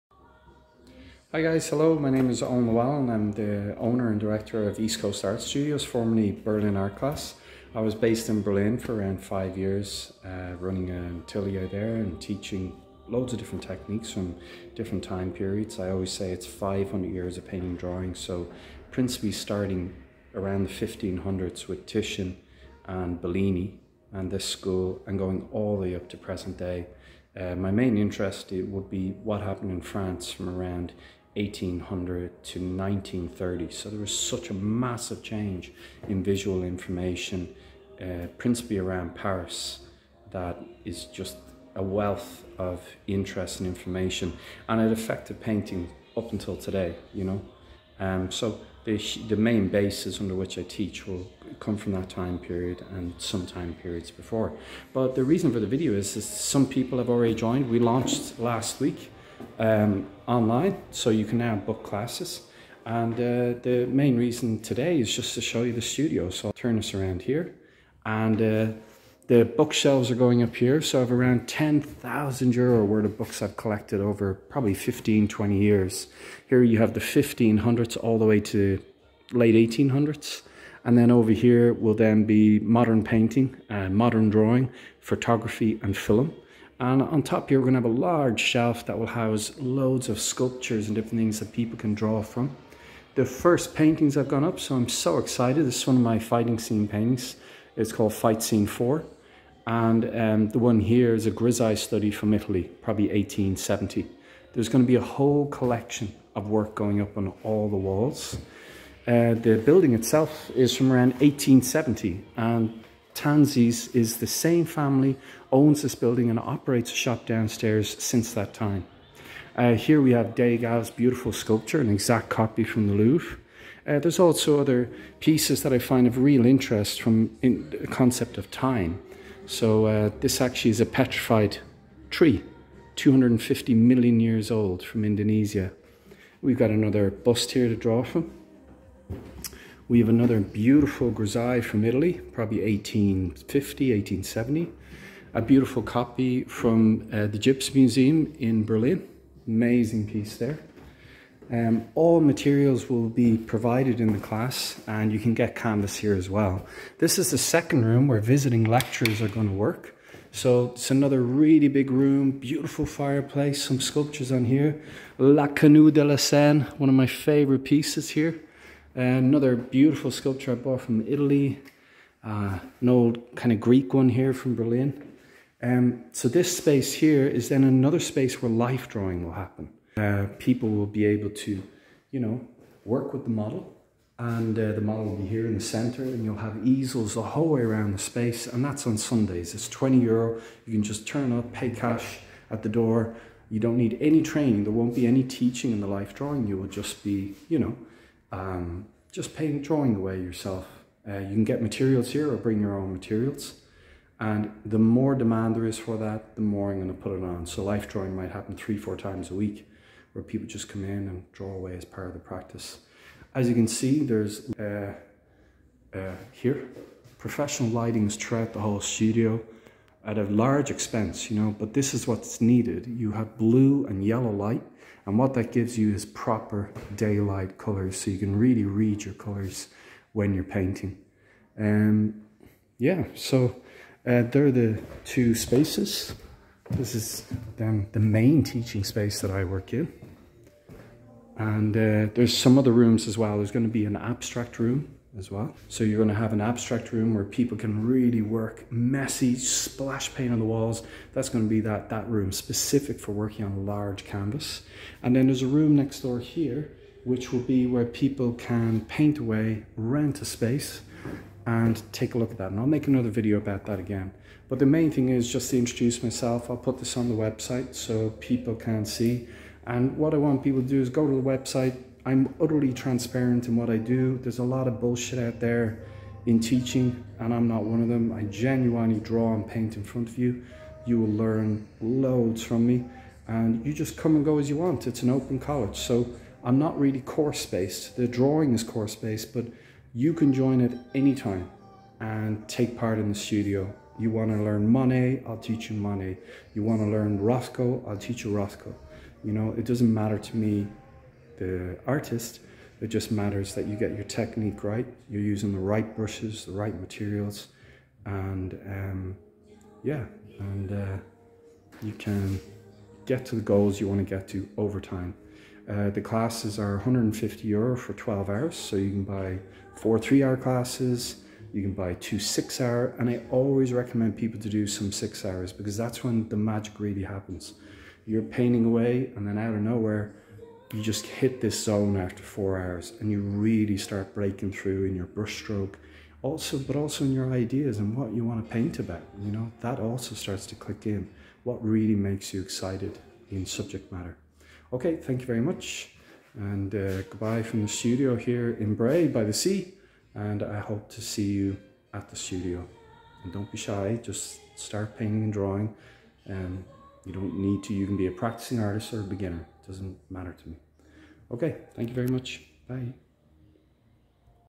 East Coast Art Studios. Library and Easels being Installed.
So excited to finally be building my studio in Ireland. I moved from Berlin in 2018 and between 1 thing and covid we are only getting to reopen now. I can't express how excited I am to get back to teaching but also getting back to creating myself.

We have a large studio 120 m2 of space. 2 teaching studios for the study of classical and contemporary art, Life Drawing with the model, 1 abstract painting room for throwing paint around, 1 large 25 m2 room for artists to have studio shared space, with 4 other artists - this can be rented by the week. A huge library, loads of high quality prints to study from, sculptures to draw and paint from, and a collection of art from the 1600s to present day. Come and study with us 5 minutes from Bray Train Station at 20 Quinsborough Road.